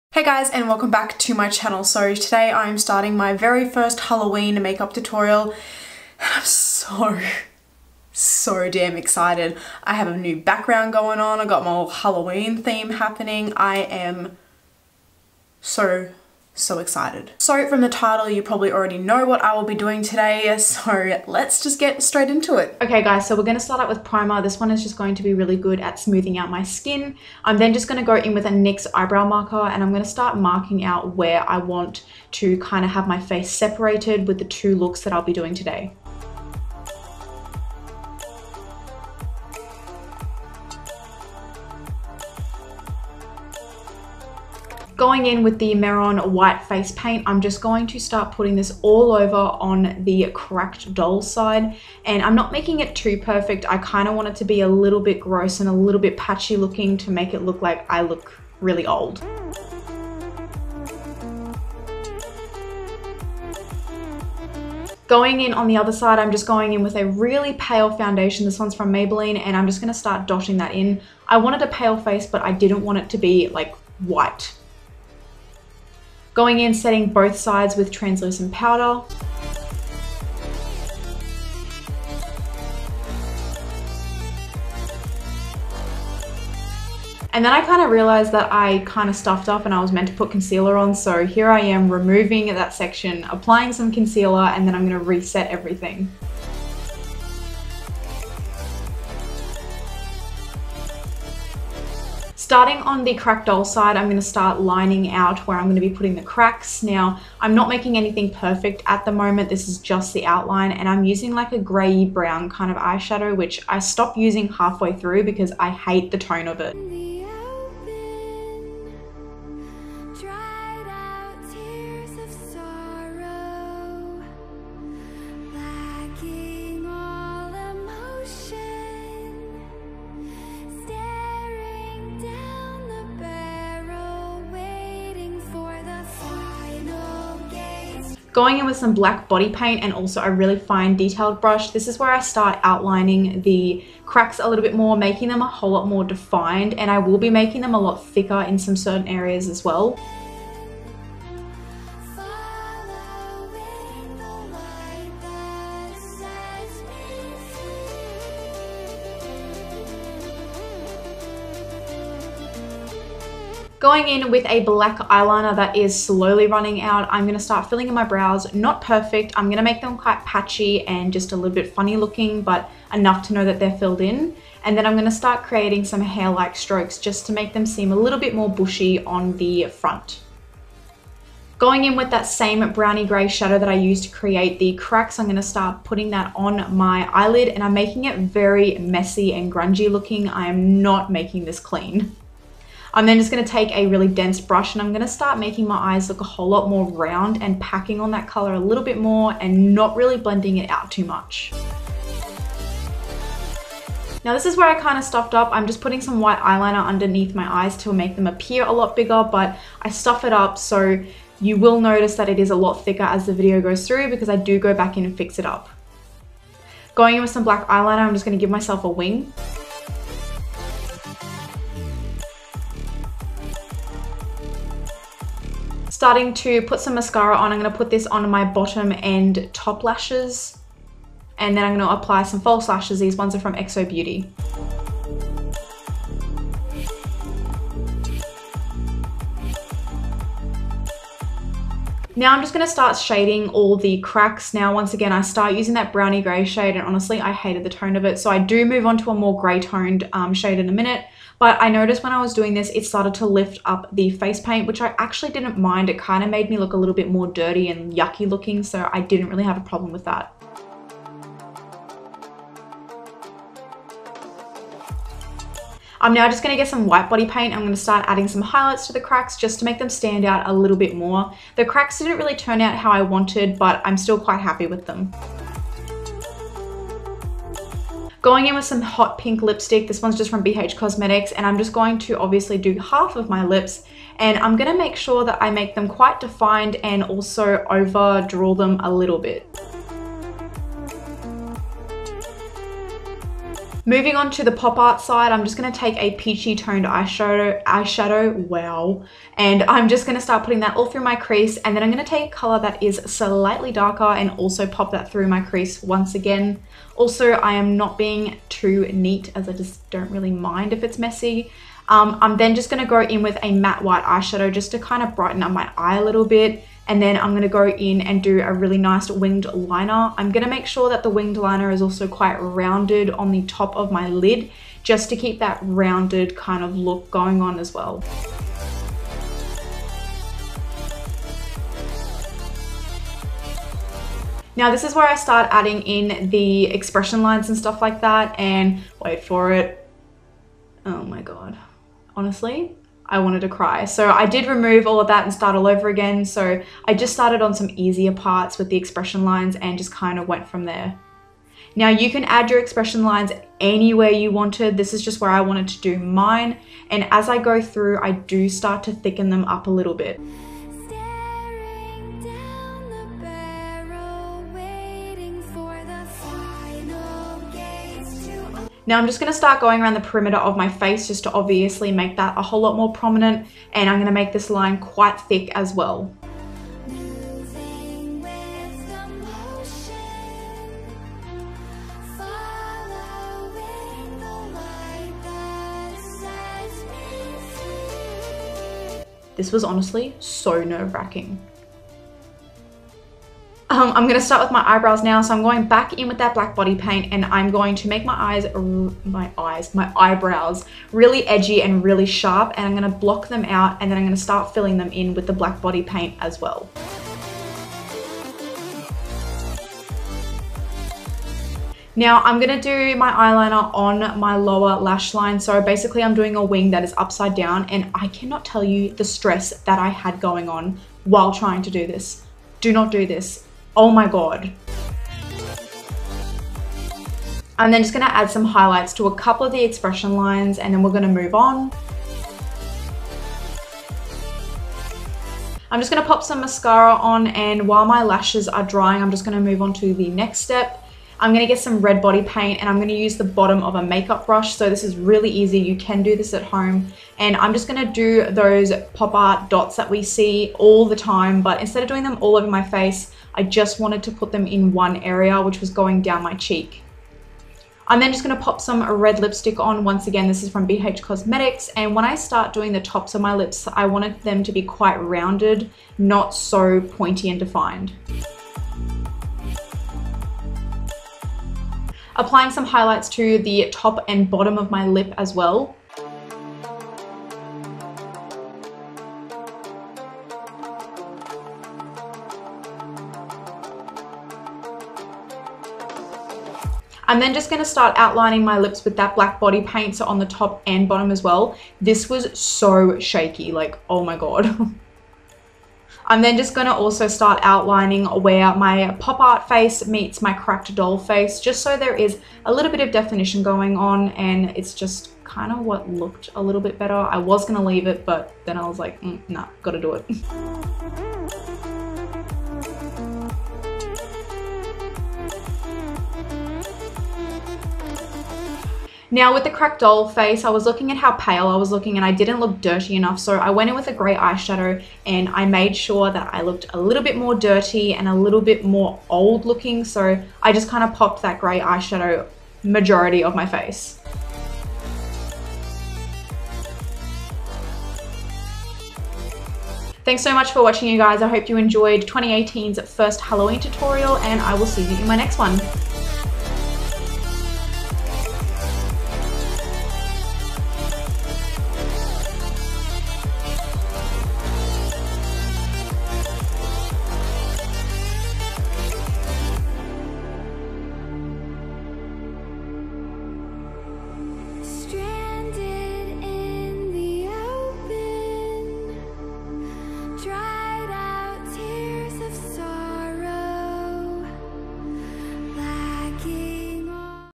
CRACKED POP ART | HALLOWEEN MAKEUP TUTORIAL | Monique Raineri
Hey guys, I hope you enjoy my first Halloween tutorial for 2018

PREVIOUS VIDEOS